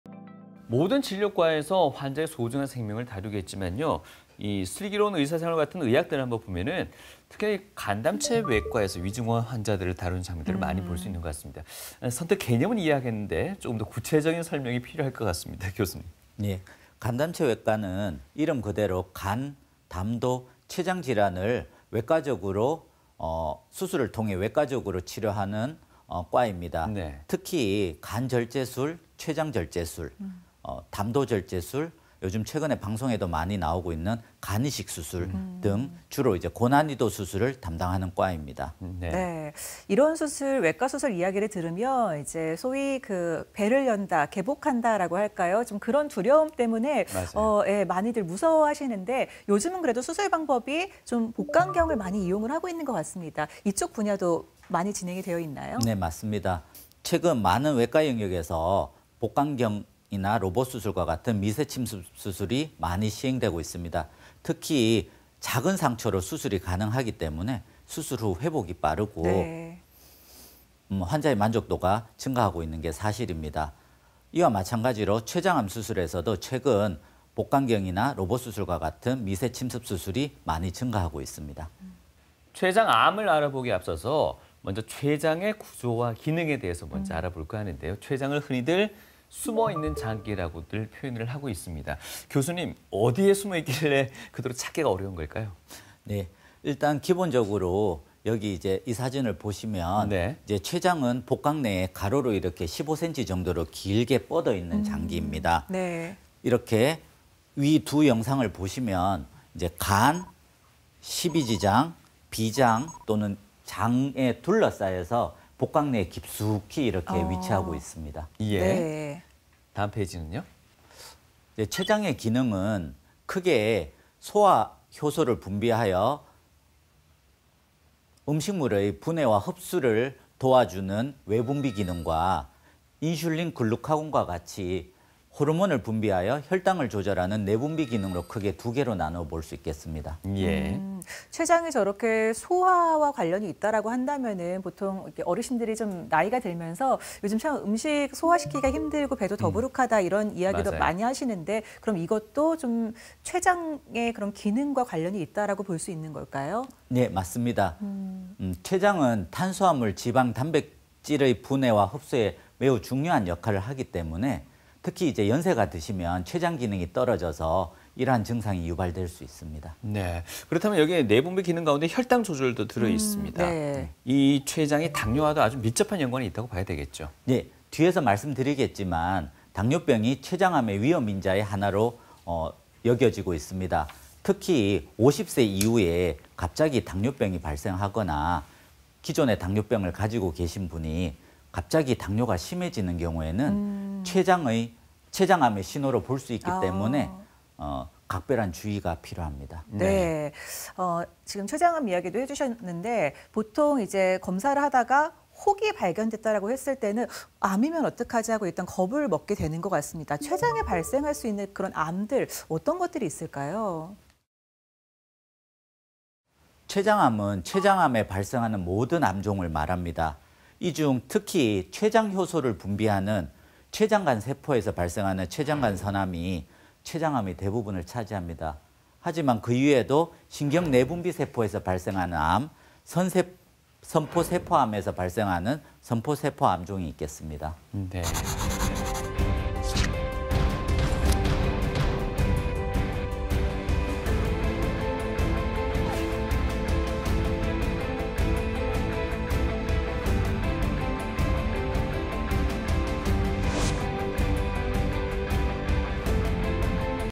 암 중 무서운 암, '췌장암' ①췌장의 구조, 기능 | 메디컬 약손
췌장암을 알아보는 첫 번째 과정은 췌장의 위치와 구조 그리고 기능...구조와 위치로 인해 복강경, 로봇수술도 가능